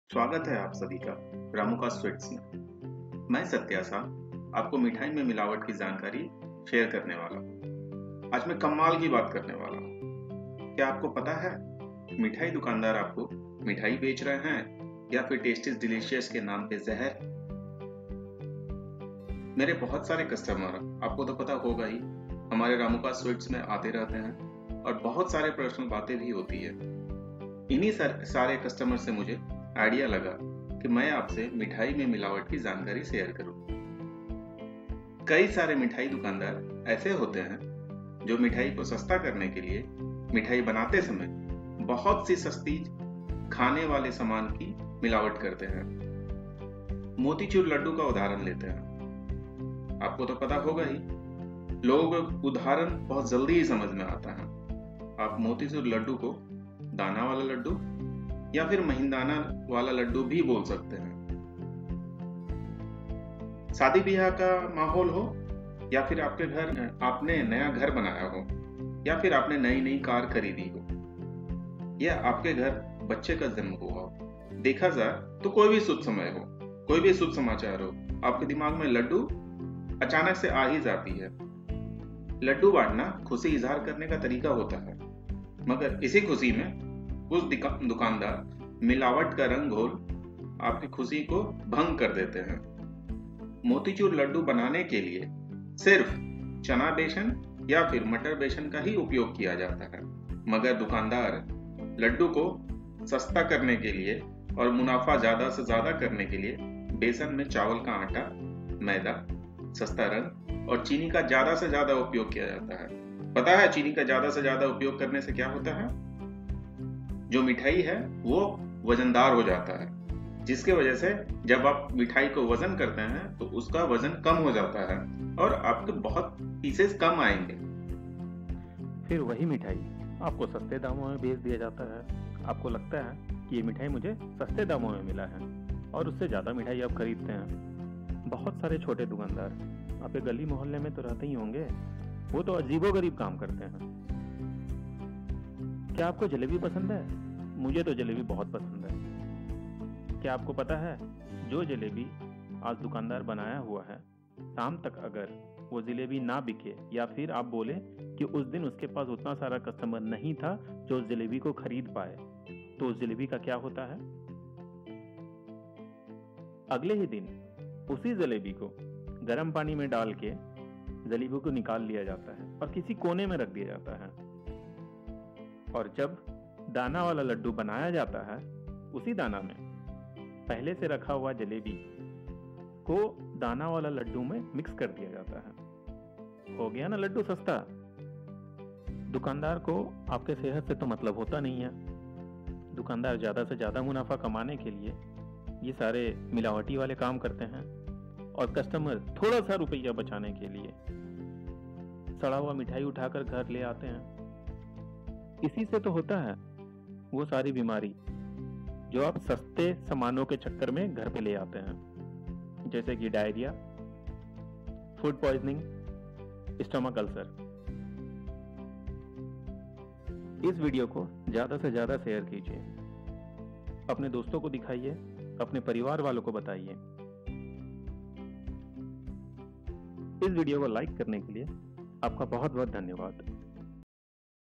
Is Mithai Shop Ke Ghotale Ne Subke Hosh Uda Diye
In this Video Ramuka's Sweets  talks about Health Code voilations That has been Taken Place in Sweets Manufacturing Business and how customers are getting sick by eating Mithais.